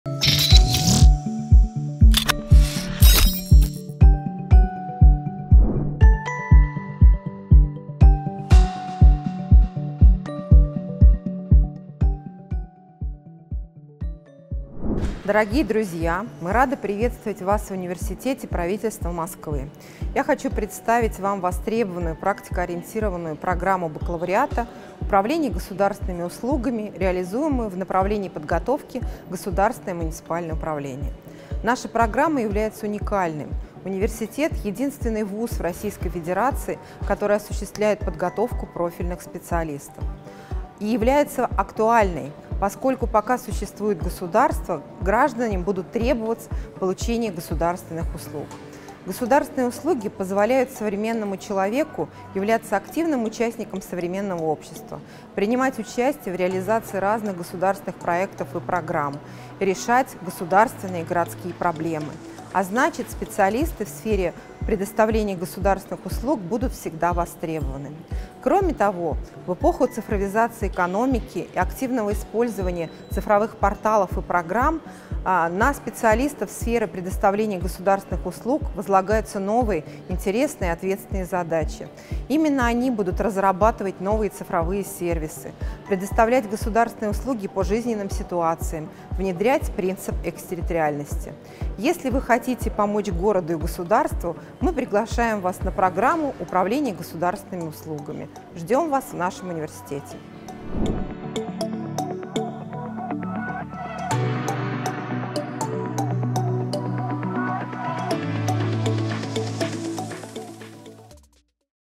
Программа бакалавриата "Управление государственными услугами" в Университете Правительства Москвы
Заместитель заведующего кафедрой государственного управления и кадровой политики Ольга Николаевна Любина о программе бакалавриата "Управление государственными услугами"

Приемная комиссия:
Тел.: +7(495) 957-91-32
Адрес: г. Москва, ул.Сретенка, 28.